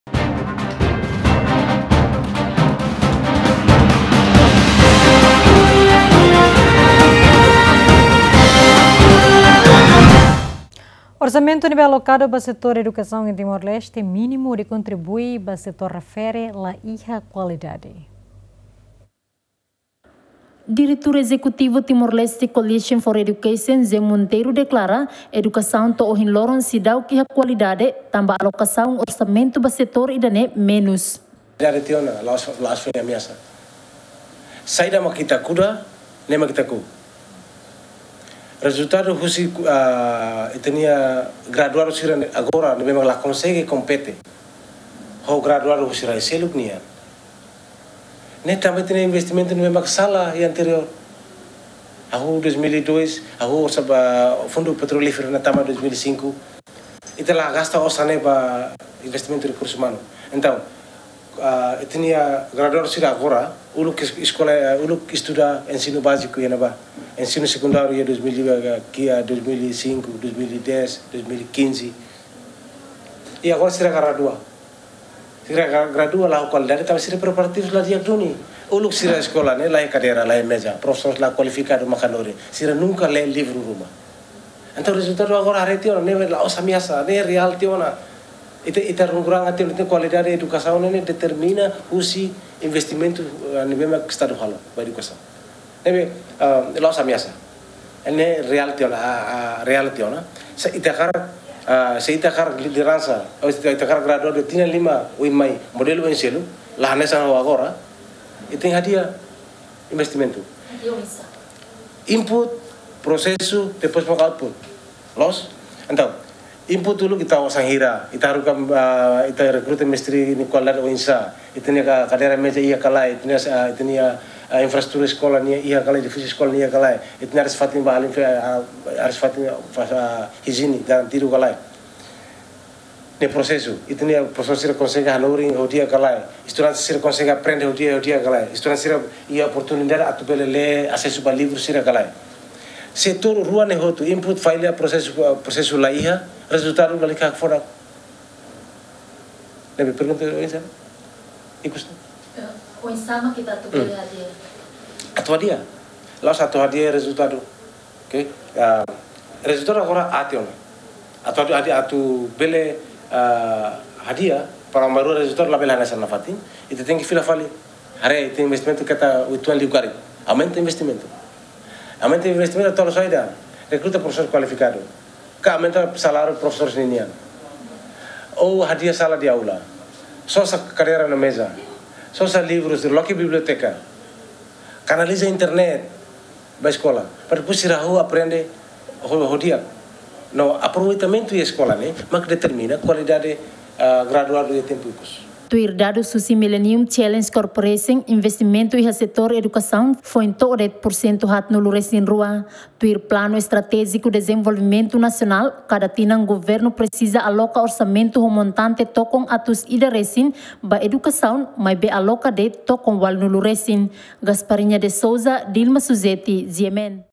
Orsamentu neebé alokadu ba Setór Edukasaun mínimu hodi kontribui ba setór refere laiha kualidade
Orsamentu neebé alokadu ba setór edukasaun iha Timor-Leste mínimu hodi kontribui ba setór refere laiha kualidade